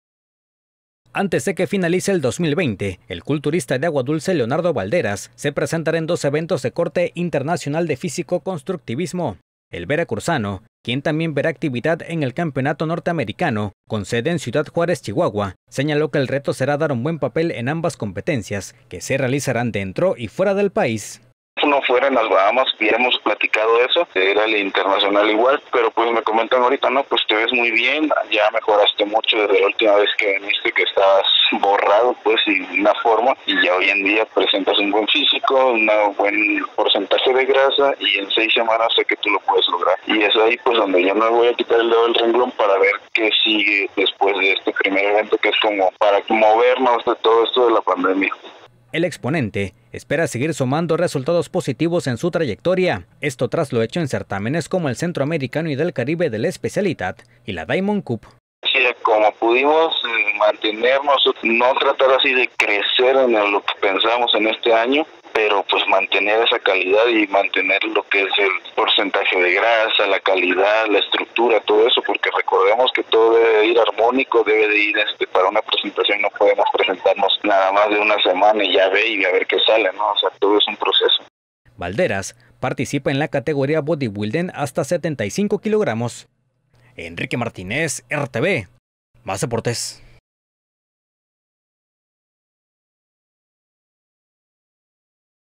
Leonardo Balderas se presentará en dos eventos de corte internacional de fisicoconstructivismo
Antes de que finalice el 2020, el culturista de Agua Dulce Leonardo Balderas, se presentará en dos eventos de corte internacional de fisicoconstructivismo.